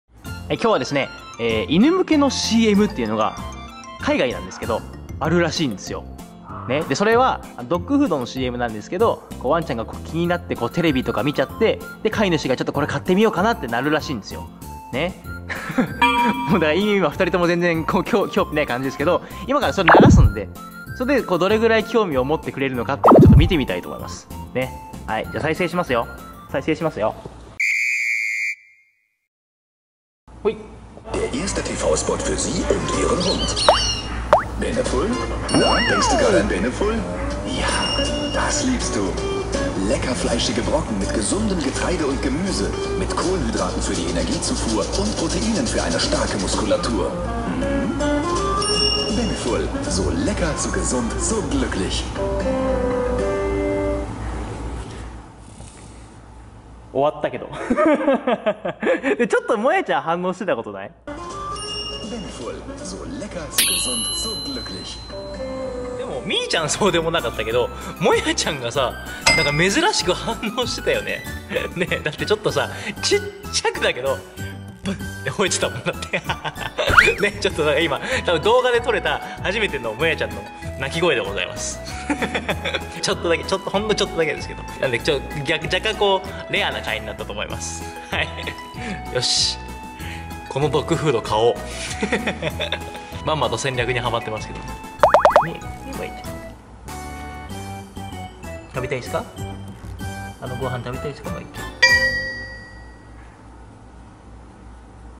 犬向けのCM！？うちのブサカワ犬に見せたら反応にビックリ！！
もやちゃんが（めっっちゃちょっとだけど）吠えるなんて！！

ちなみに見せたのはこのCMです。
もやちゃんは「シャーペイ」という犬種です。シャーペイは、世界一珍しい犬としてギネスブックに載っています。
おサルさんとこんなことしてほしいっていうリクエストもお待ちしております！